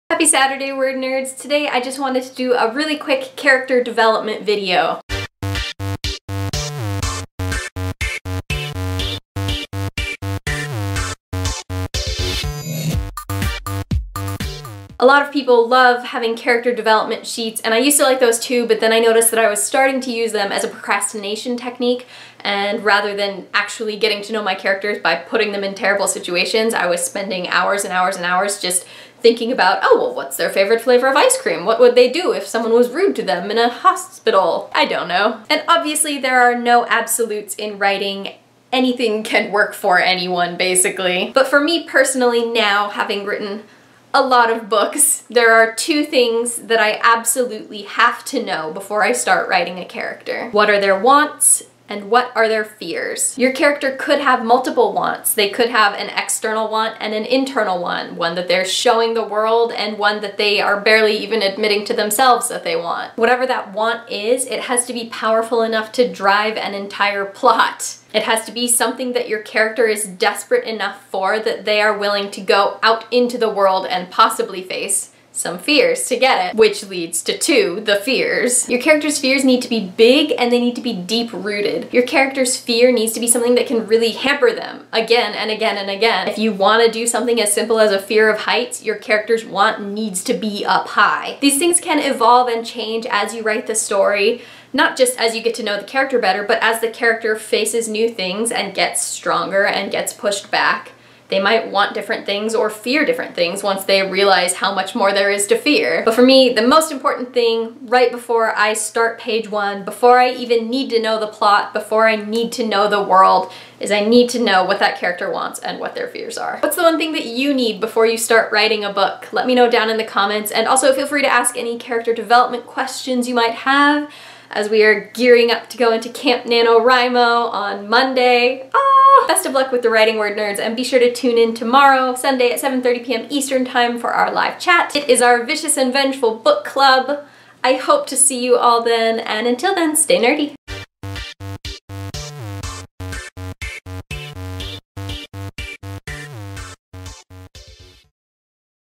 WANTS and FEARS
Meghan gives her one pre-writing must have. What's yours?

SUNDAY SPECIAL LIVE CHAT TOPIC: Book Club - VICIOUS and VENGEFUL by V.E. Schwab